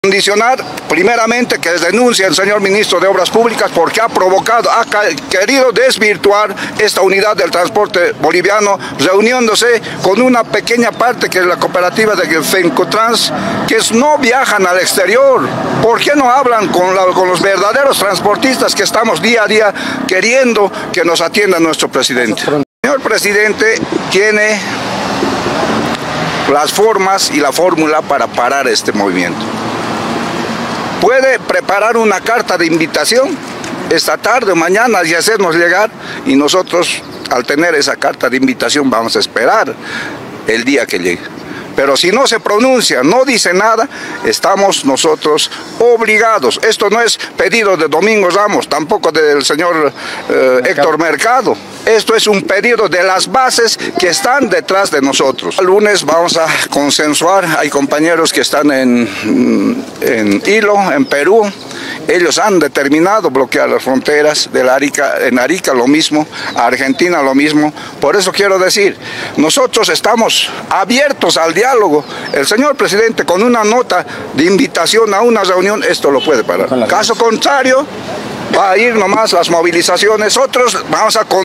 TRANSPORTE PESADO RATIFICA MOVILIZACIONES Y EXIGEN LA RENUNCIA DEL MINISTRO MONTAÑO
El dirigente del Transporte Pesado ratificó las movilizaciones de su sector y aseguró que solo se reunirán con el presidente, ya que el ministro Montaño los menospreció y se reunió con transportistas que no son internacionales.